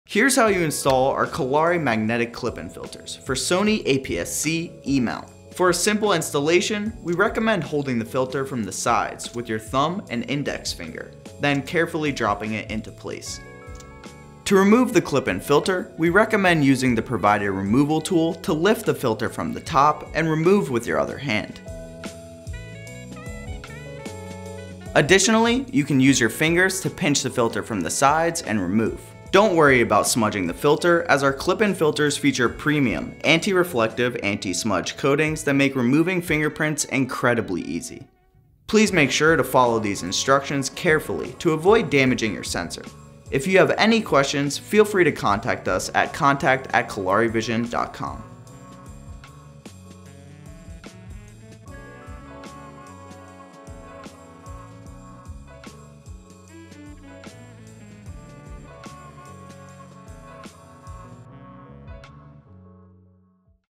Kolari Magnetic Clip-in Filter For Sony APS-C E | Installation Tutorial
Here is how to install our new Kolari Magnetic Clip-in Filters for Sony APS-C E-Mount! Compatible with most Sony mirrorless crop sensor systems.

These filters consist of the professional-level optics we’re known for securely fitted into a CNC-machined aluminum magnetic frame, designed with maximum lens compatibility in mind. This one size fits all filter solution also works great with lenses that don’t have filter threads and can also be used alongside external lens filters for multiple layers of filtration.

We apply premium anti-reflective and anti-smudge hard coatings to our premium glass to increase light transmission while reducing flares and ghosting. Hydrophobic, dust resistant, and scratch-resistant, these multi-coatings make cleaning and upkeep an effortless task.

Filter Options:
ND Filters
Mist Filters
Infrared + more